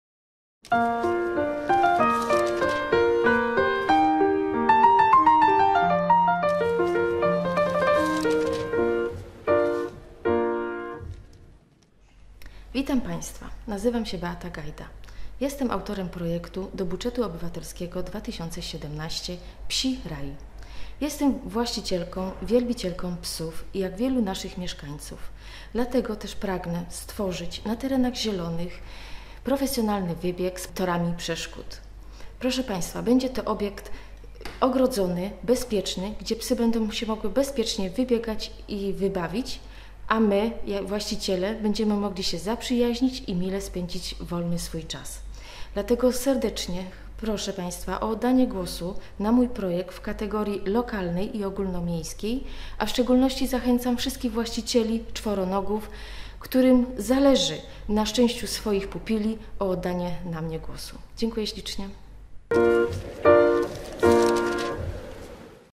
BUDŻET OBYWATELSKI 2017 - SIEMIANOWICE ŚLĄSKIE
Ogólnomiejski i Lokalny Budżet Obywatelski 2017, wniosek nr 4 i 9
„Psi Raj” - wybieg dla psów